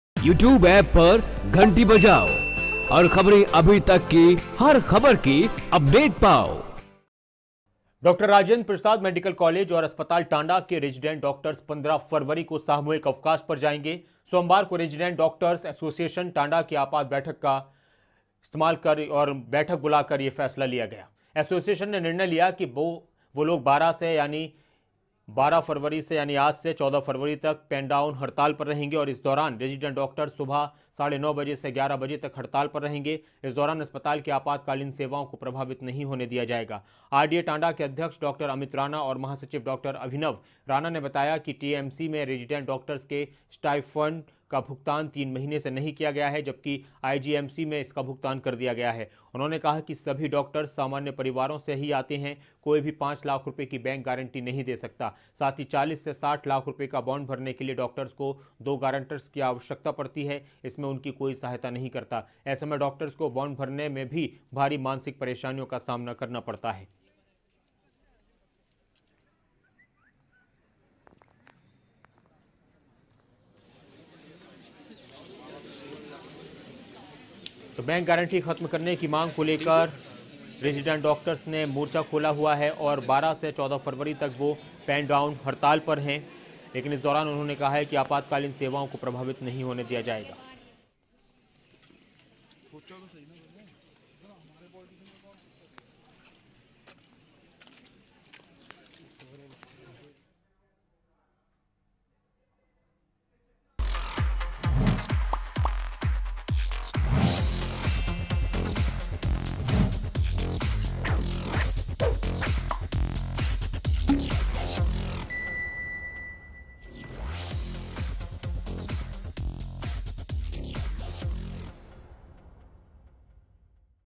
टांडा मेडिकल कॉलेज के डॉक्टरों ने सरकार के खिलाफ खोला मोर्चा || Khabrain Abhi Tak Live
डॉक्टर राजेंद्र प्रसाद मेडिकल कालेज टांडा में बैंक गारंटी के विरोध में रेजिडेंट डॉक्टरों ने सरकार के खिलाफ मोर्चा खोल दिया.. डॉक्टरों ने दो घंटे की पेन डाउन स्ट्राइक शुरू की... और सरकार से जल्द बैंक गारंटी खत्म करने की मांग की है... हालांकि सरकार ने हाल ही में बैंक गारंटी को दस लाख से घटा कर पांच लाख किया है... लेकिन डॉक्टर इसे पूरी तरह से खत्म करने पर अड़े हुए हैं... डाक्टरों की अंशकालिक पैन डाऊन हड़ताल के चलते सुबह के समय मरीजों को परेशानी उठानी पड़ी...